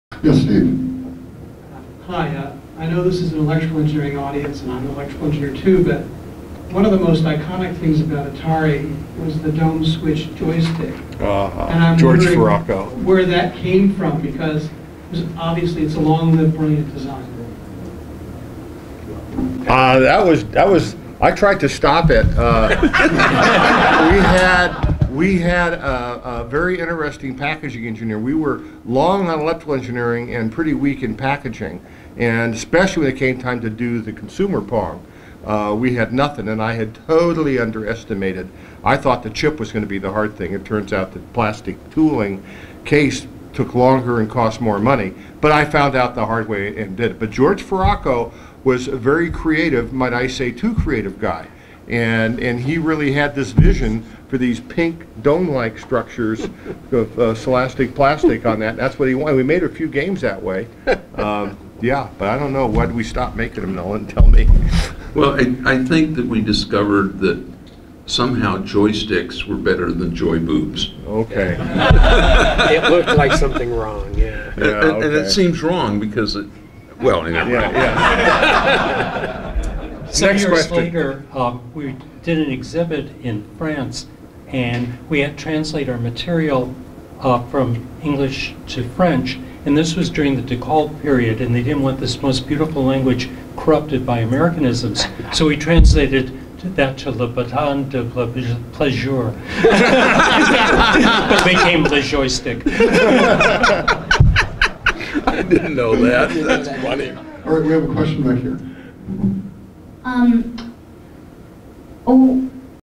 Origin of Joystick
The panelists talk about how they came up with the joystick.